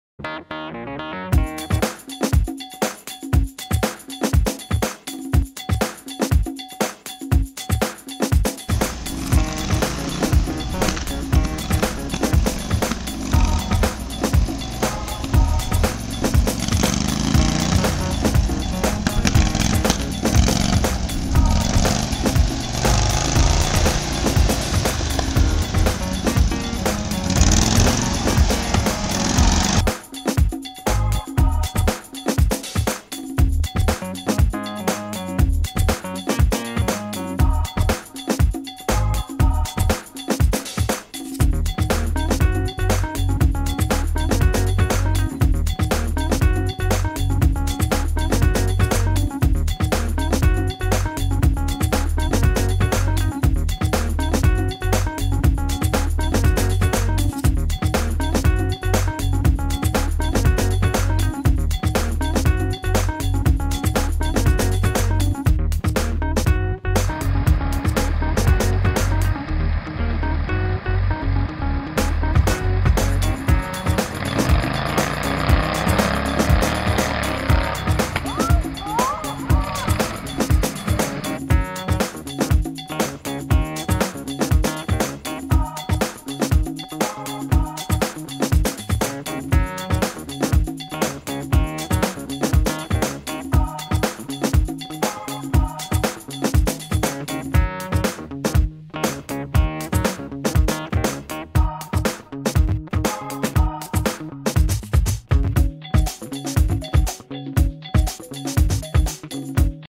The Narwhal!
Ok so for family and friends here is our little video before race day of our drag mower in action. We had so much fun as a family doing this so many laughs this was so over the top. Now we are off to race against three other teams. We will have a drag race, tug war, and a weight pulling race.
1. "The Yard Man" (Team Hoy)
2. "The Mow Foe" (Team Hoy, Leon, & Braun)
3. "The Holy Rollers" (The Henderson Family)